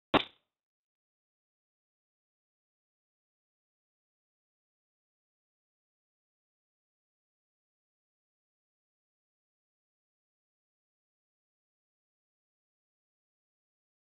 CELTECH Dosage pistolet patisserie
Doseuse à piston CELTECH modèle CEDEP, application patisserie avec pistolet coupe-goutte et bras support